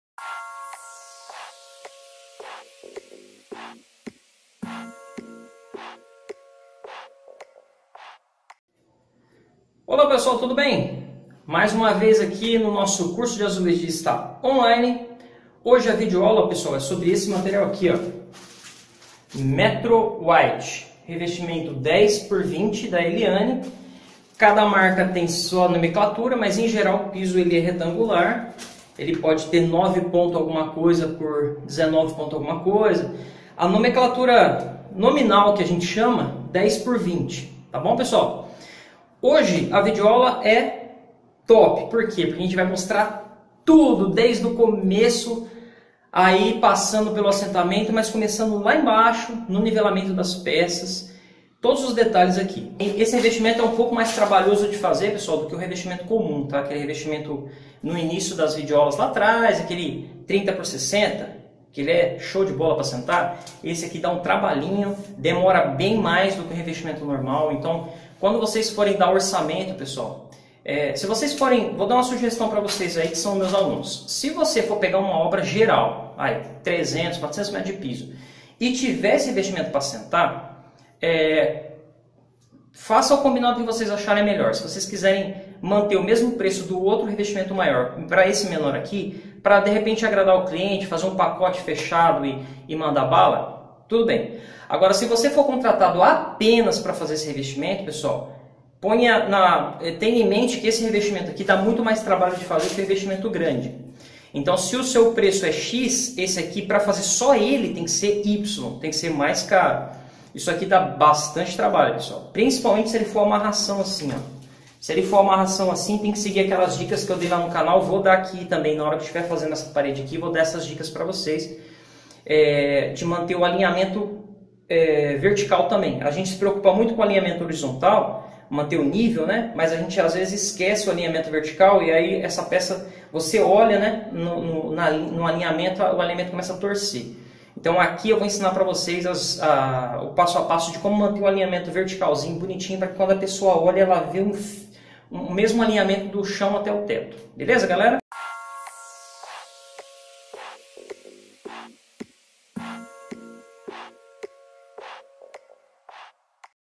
Qual valor cobrar no assentamento de revestimento pequeno,como pastilhas ou retangulares.
Qual valor cobrar no assentamento de revestimento pequeno,como pastilhas ou revestimentos retangulares.

Essa é mais uma dica do curso de azulejista on line.
Venha fazer parte da nossa equipe de azulejistas espalhado por todo mundo.
Acesse e comece agora watsapp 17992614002